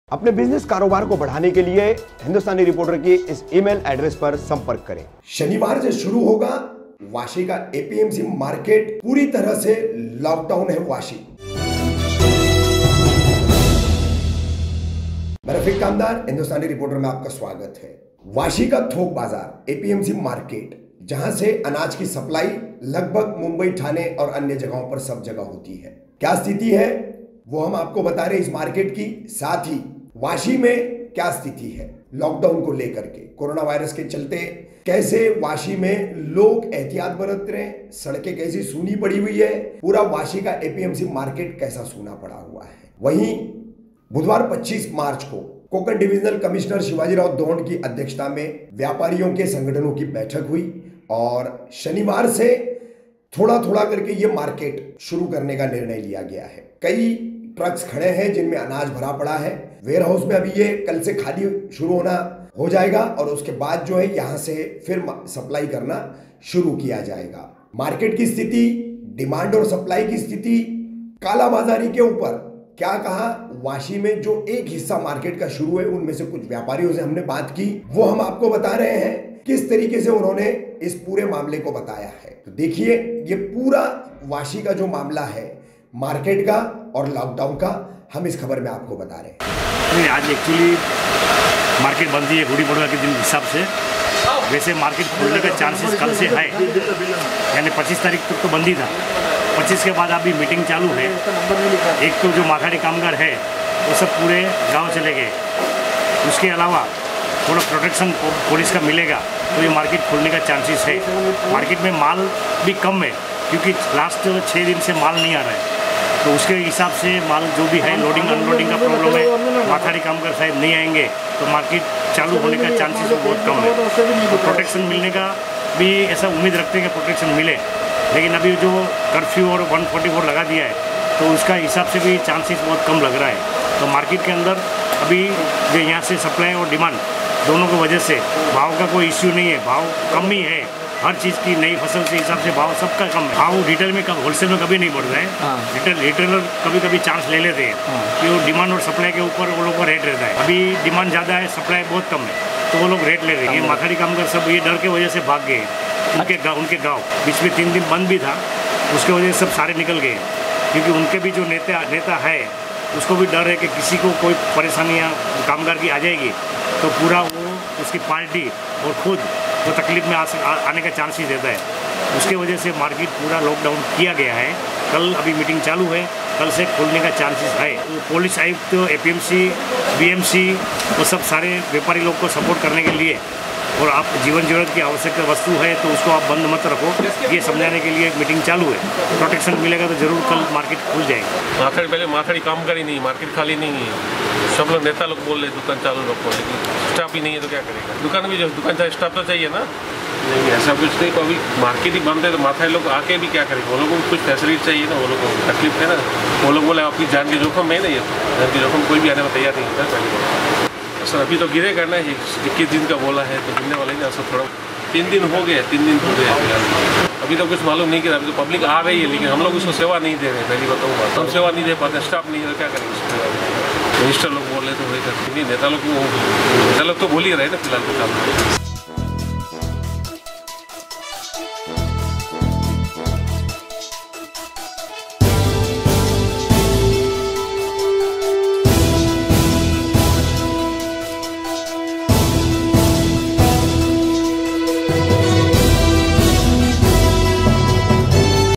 बंद पडा है वाशी का एपीएमसी मार्केट | Hindustani Reporter
बंद पडा है वाशी का एपीएमसी मार्केट

माथाडी कामगारो के गांव जाने से काम हुआ ठप्प

Mic We Use